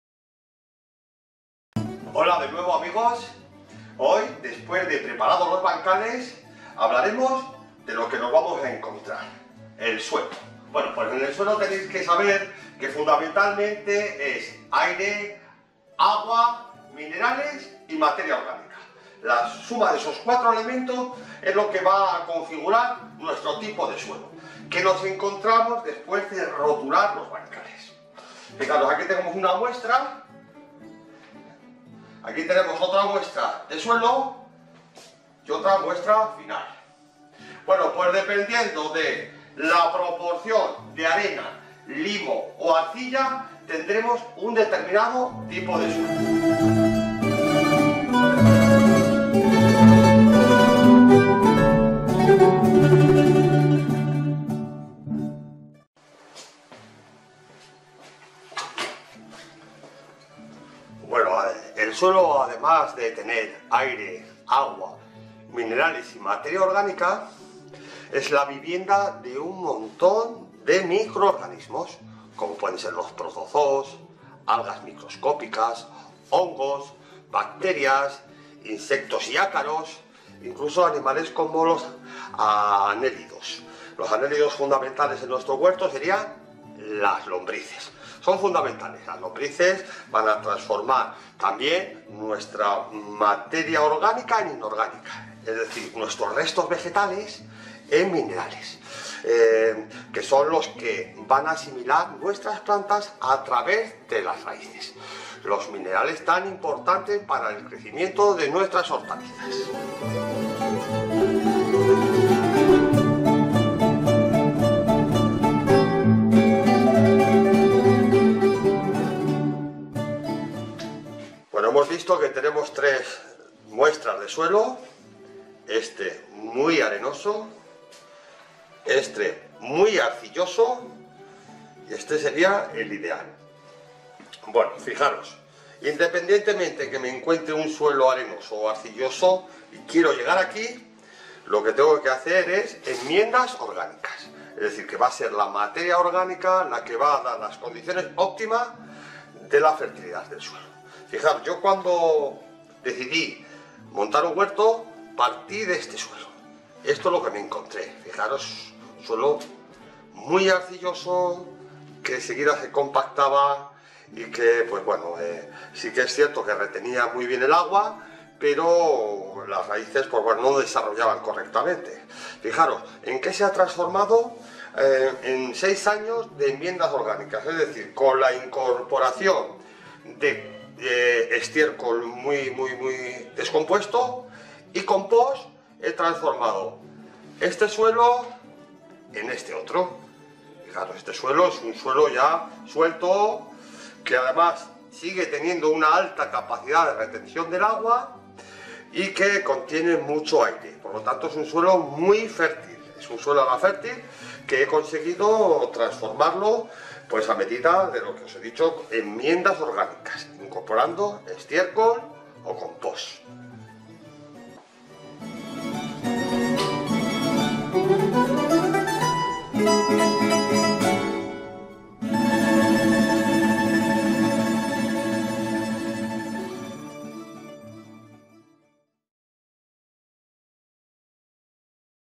Huerto ecológico - 3º entrega - El Suelo
Josu, el educador ambiental de la Casa del Parque de Montes Obarenes - San Zadornil, que se encuentra en la localidad burgalesa de Oña, sigue con su curso para crear y cuidar un huerto ecológico. En este tercera entrega nos habla de los suelos.